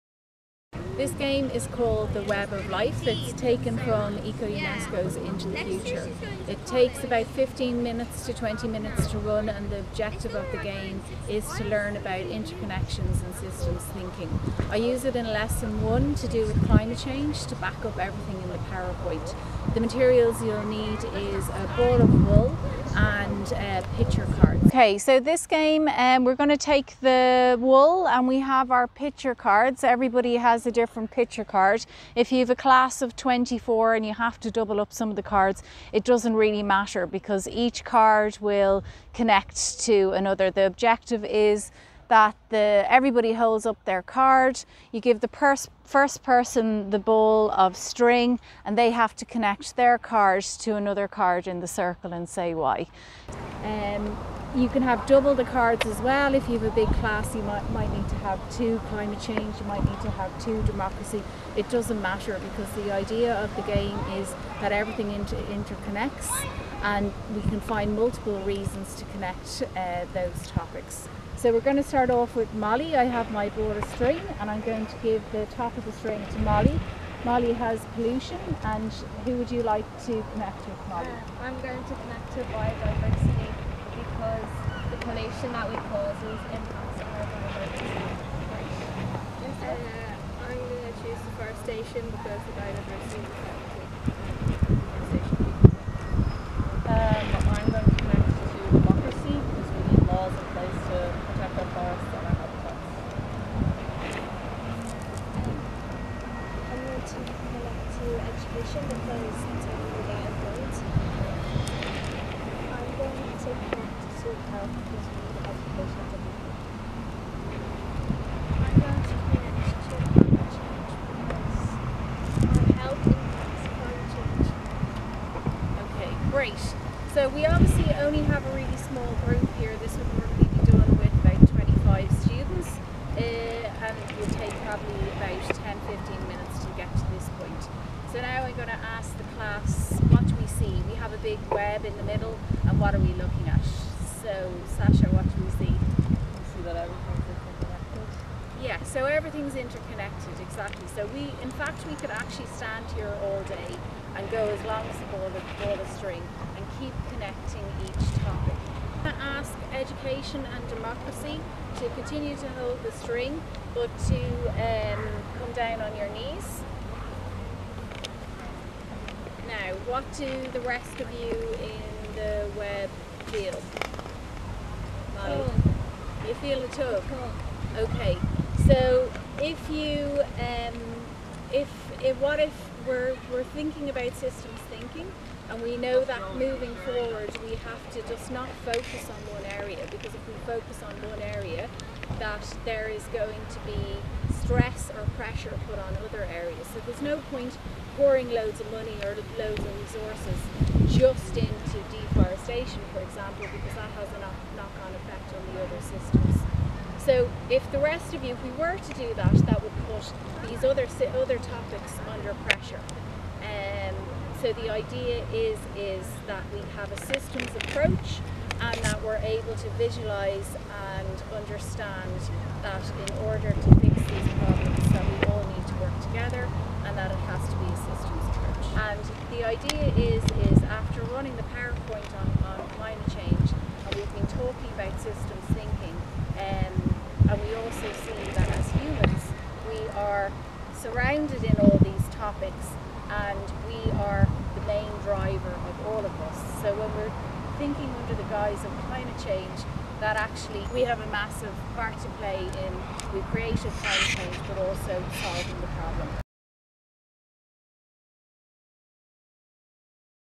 ACTIVITY: Web of Life
Source: Into the Future/ ECO UNESCO
Materials: picture cards and a ball of string
Time: 15 minutes
This game supports a lesson on climate change and systems thinking.
The learning objective is to understand systems thinking and interconnections.

Method:
- Ask students to stand in a circle facing each other holding up their card so the rest of the group can see it.
- Give the ball of string to one participant and ask them to hold the end of the string. This participatn then passes the ball to someone in the group that holds a card that connects with their card in some way.
- The next person in turn passes the ball of string to a different person/card that their card connects to.
- This continues until all participants are linked and a web of string is created inside the circle.
- Ask the students to reflect on what they see. Discuss that everything is connected and therefore we need a systems approach to sustainable development and to climate solutions.